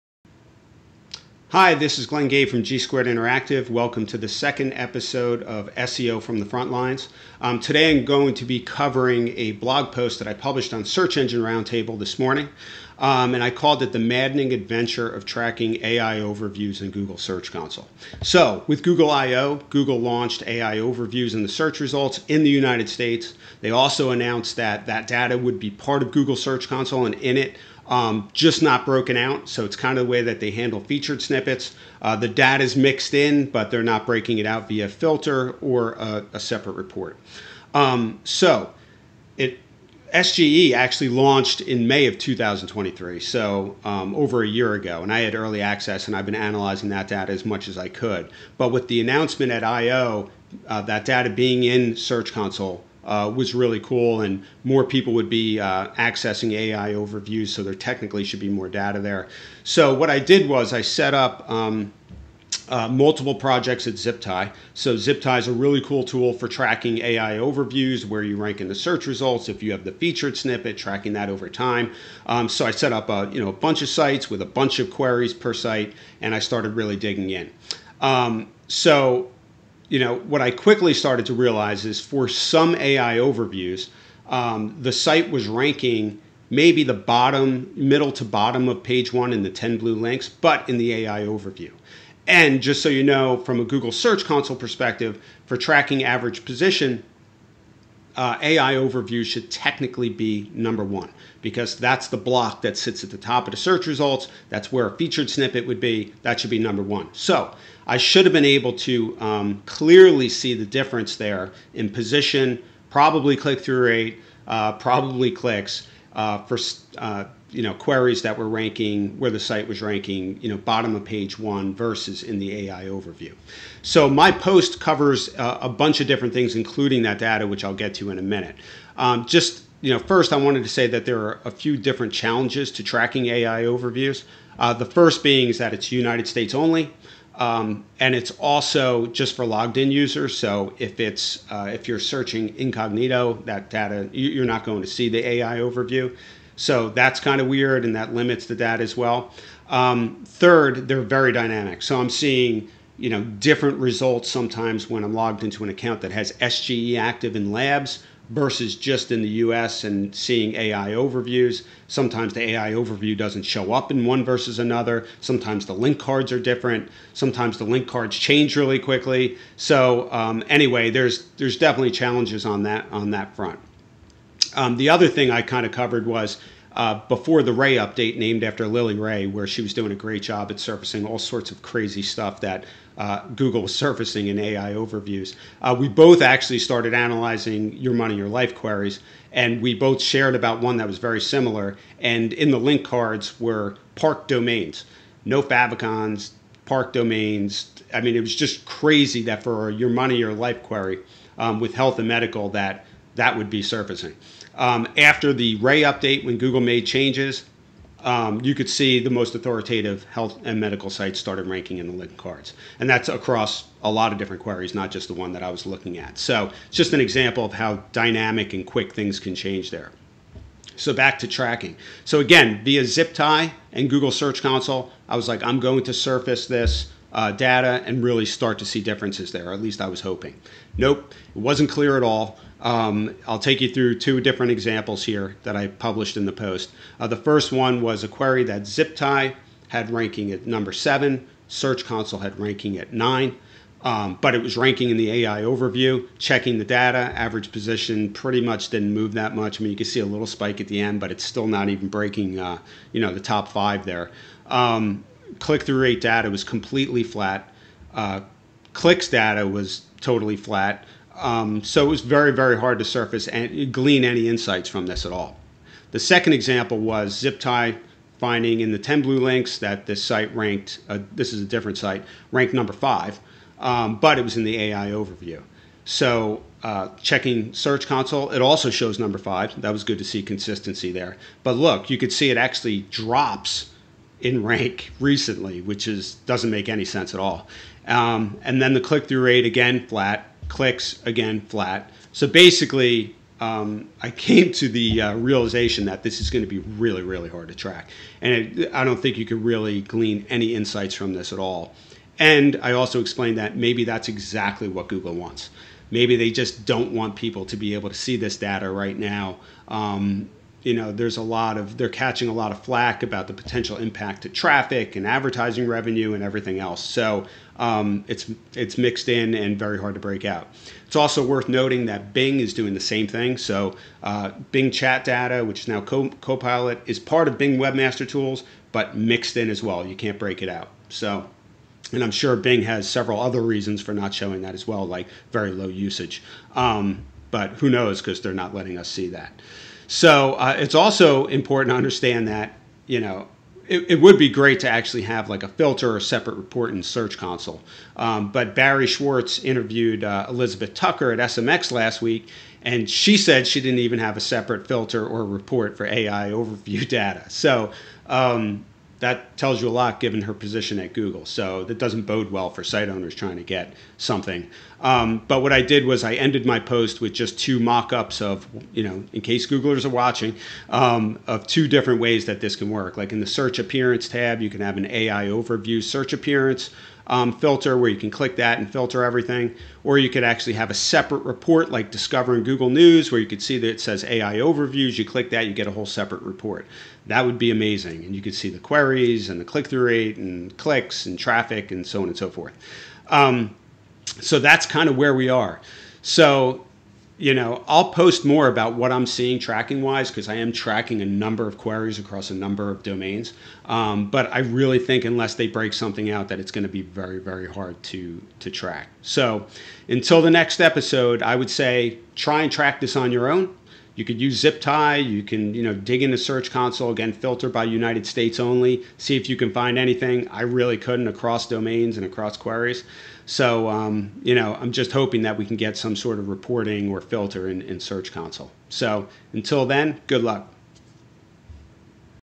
Tracking AI Overview data in Google Search Console (GSC) - Case Study
Now that AI overviews rolled out in the United States, I dug into Google Search Console to track the change in clicks, click through rate, and more. I walk through several examples of sites ranking lower on page one in the 10-blue links, but also ranking in AI overviews. I was hoping that I would clearly be able to see differences there when that was the case. Nope, not clear at all.

My post on Search Engine Roundtable covering tracking AI overviews in Google Search Console